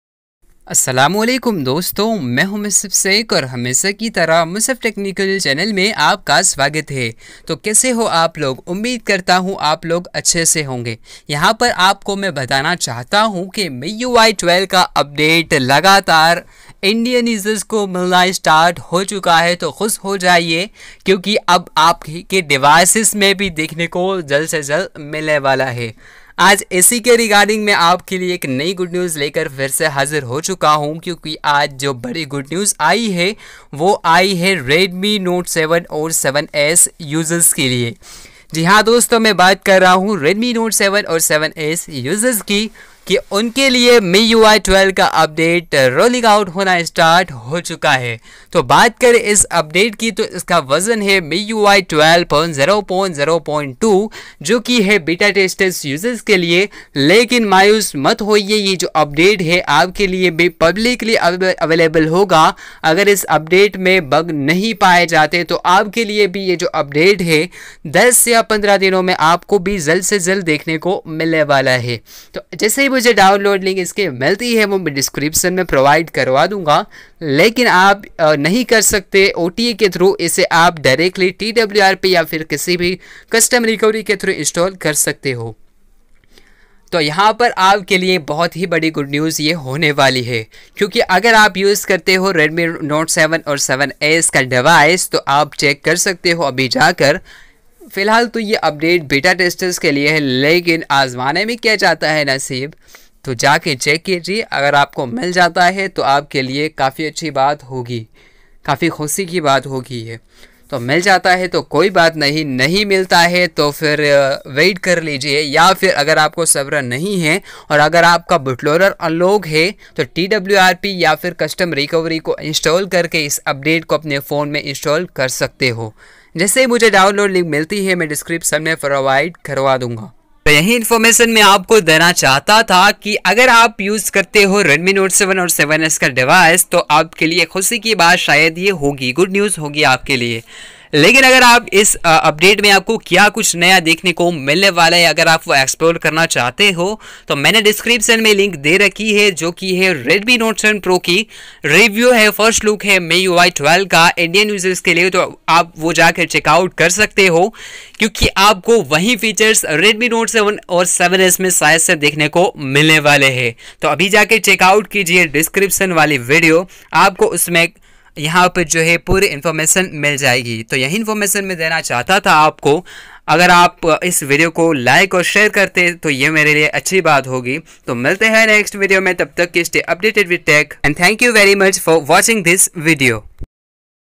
Redmi Note 7/7S MIUI 12.0.0.2 India Stable Update Rolling Out Start | Download Link
This video is about Redmi Note 7/7S MIUI 12 Update with Android 10 for Indian users. yess, you heard it right. Finally the wait is over. Most awaited update for Redmi Note 7/7S is now started to rolled out. Thus, this Update is for stable beta testers or selected users only. The version of this update is MIUI 12.0.0.2 QFGINXM it is said that this update is available soon for public release. But if you can't wait and want to experience MIUI 12 India Stable Update in your Redmi Note 7/7S then here is the download link :

Recovery ROM (Flash Only By Custom Recovery)

Stay Updated With Tech.